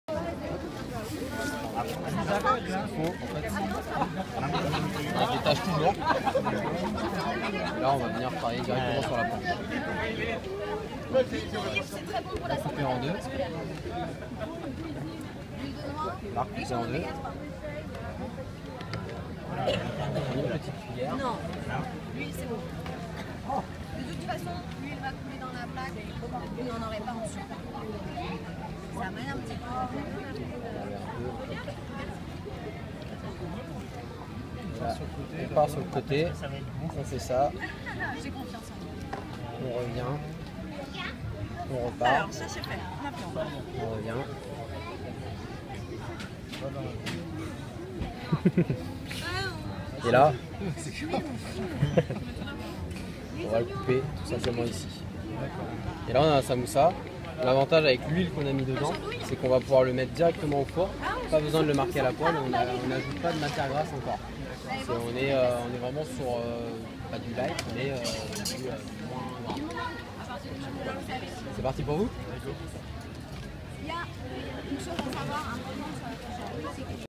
Recette de samoussas à Parlons Terroir à Mantes la Jolie
Au marché gourmand Parlons Terroir à Mantes la Jolie, des cours de cuisine étaient donnés au public présent par un chef.